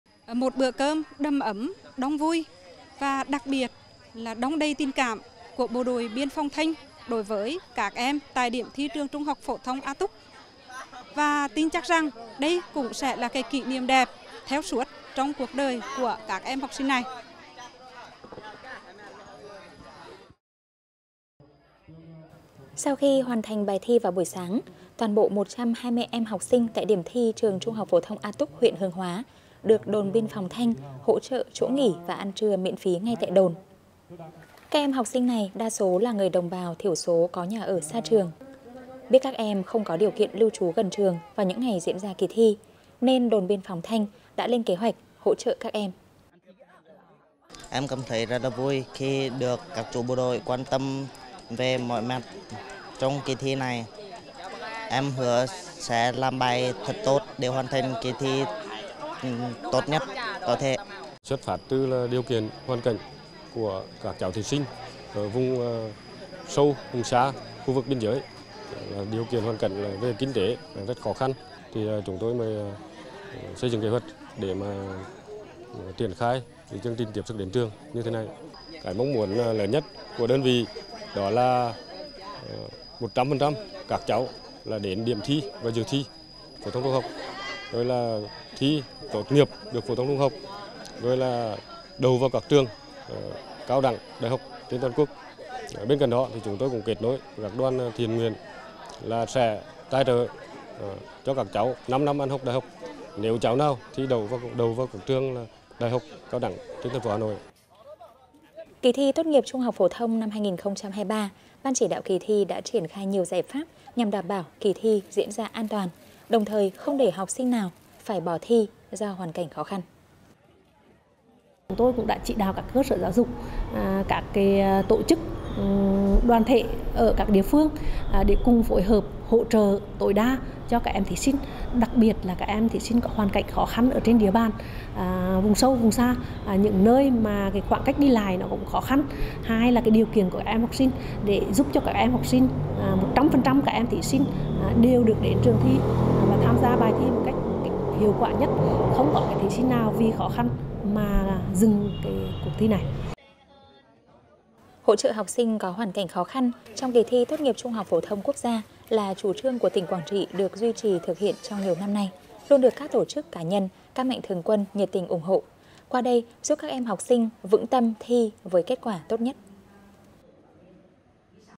Tiếp sức mùa thi ở vùng sâu, vùng xa-Truyền Hình Quảng Trị
Với đặc thù của các huyện vùng cao, giao thông đi lại khó khăn, các đơn vị đóng chân trên địa bàn đã phối hợp với cơ quan chuyên môn thường xuyên nắm bắt tình hình, hỗ trợ cho các em học sinh khó khăn, quyết tâm không để học sinh bỏ thi tốt nghiệp THPT. Trong đó, Đồn biên phòng Thanh, BĐBP Quảng Trị đã chủ động hướng dẫn, đưa đón và hỗ trợ học sinh miền núi trong những ngày diễn ra kỳ thi.Subscribe kênh để theo dõi các tin tức mới nhất